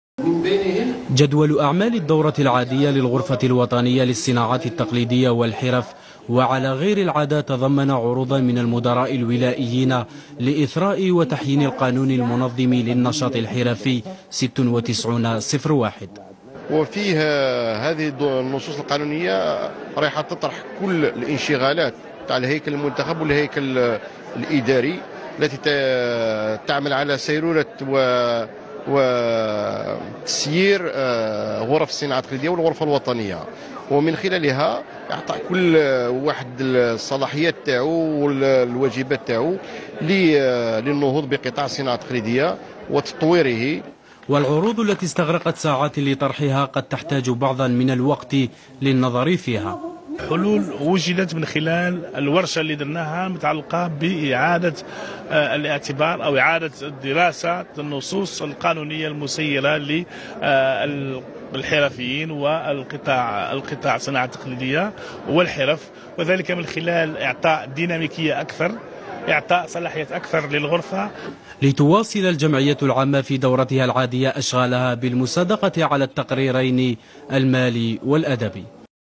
الدورة العادية للجمعية العامة للغرفة الوطنية للصناعة التقليدية
نحو إثراء قانون 96 -01 المنظم لنشاط الصناعات التقليدية والحرف
Télévision Algérienne - المؤسسة العمومية للتلفزيون الجزائري
Chambre Nationale de l'Artisanat et des Métiers - Algérie